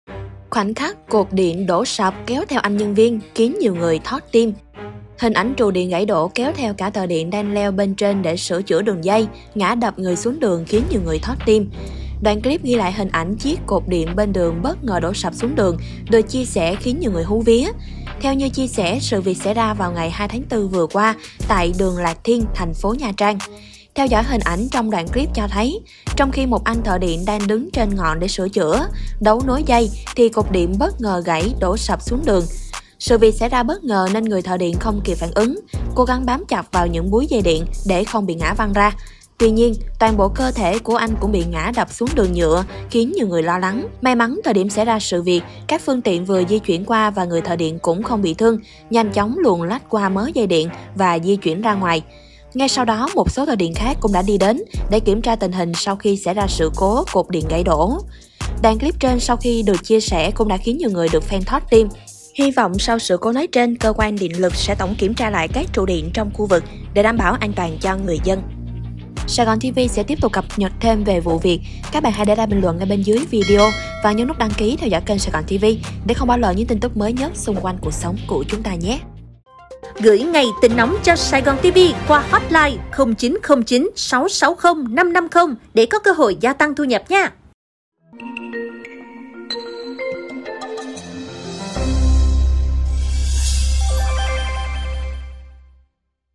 Khoảnh khắc cột điện đổ sập kéo theo anh nhân viên khiến nhiều người “thót tim” | Tin tức SaigonTV
May mắn thời điểm xảy ra sự việc các phương tiện vừa di chuyển qua và người thợ điện cũng không bị thương, nhanh chóng luồn lách qua mớ dây điện và di chuyển ra ngoài.